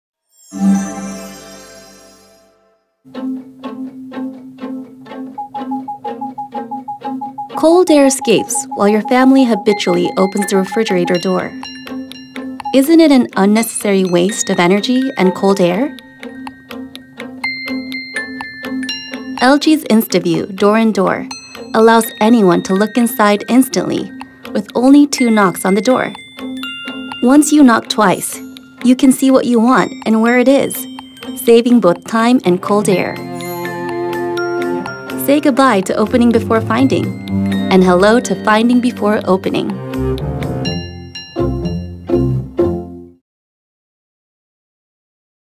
InstaView Door-in-Door™ | Knock Twice, See Inside V2
LG’s InstaView Door-in-Door™ is equipped with a sleek glass panel. Knock twice and illuminate the interior. See what is inside without opening the door!